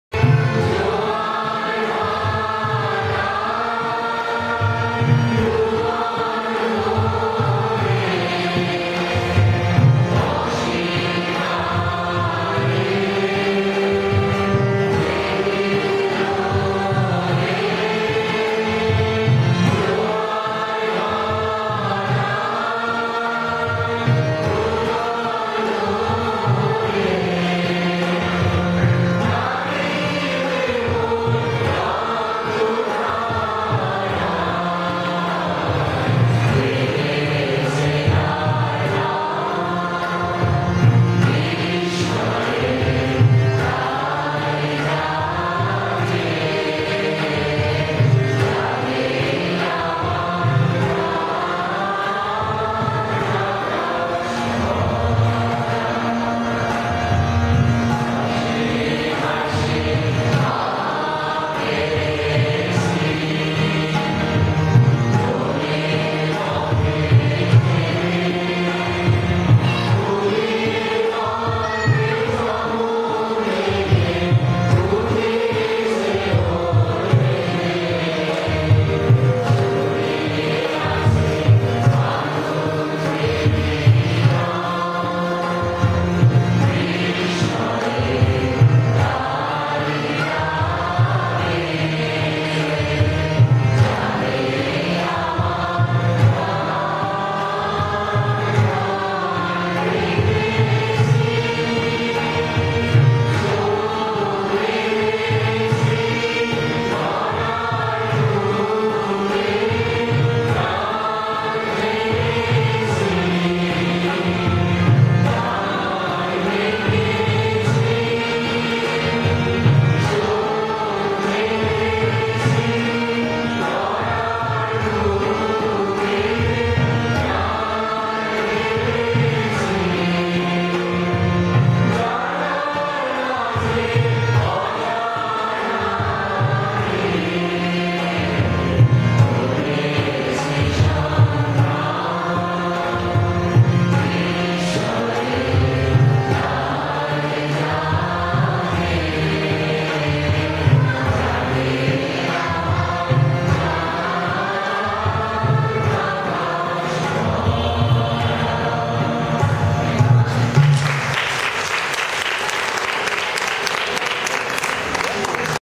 Nostalgic ambiance ..... !
A Chorus Song From a larger number singer in the audience on the competition Day organized by 74th Greater Pune Durgotsav Committee .at Bangiya Sanskriti Samsad ,Bhosle Nagar ,Pune on 15th Sept 2013 .